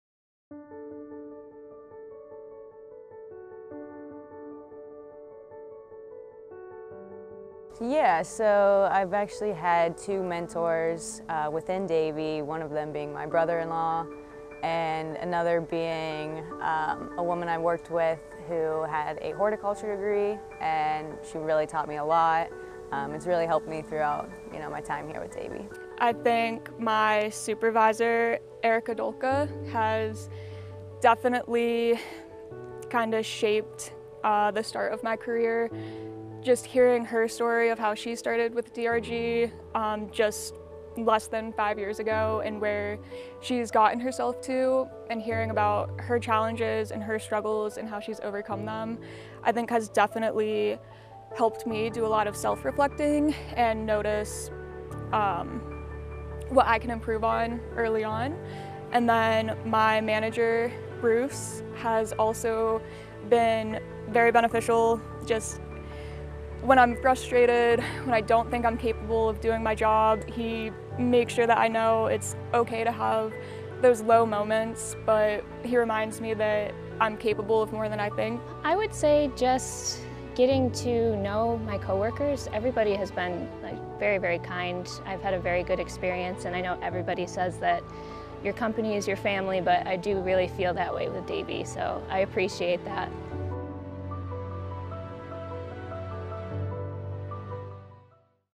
Is There a Moment or Mentor That Has Positively Shaped Your Davey Experience?
Employees who attended our Women's + Event share a moment or mentor that has positively shaped their Davey experience.